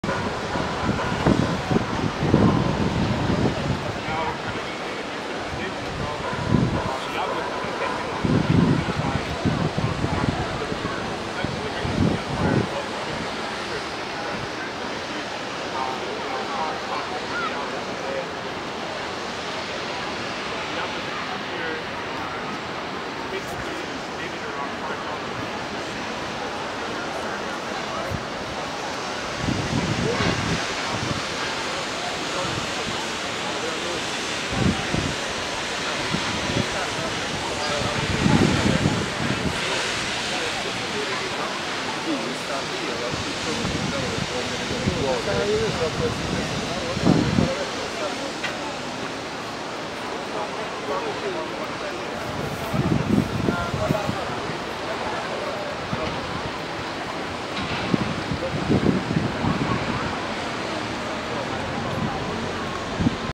Finestra del Papa! Piazza San Pietro a Roma il giorno dopo l'elezione di Papa Francesco!
Finestra del Papa!
Piazza San Pietro a Roma il giorno dopo l'elezione di Papa Francesco!